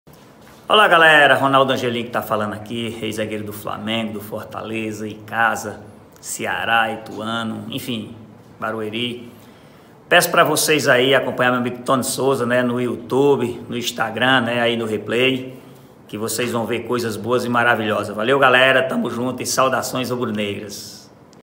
SE LIGA NO RECADO DE RONALDO ANGELIM.
O ex-zagueiro de Flamengo, Fortaleza, Icasa, Juazeiro, Ceará, Ituano e Grêmio Barueri, Ronaldo Angelim deixa um recado para você. Se liga!😉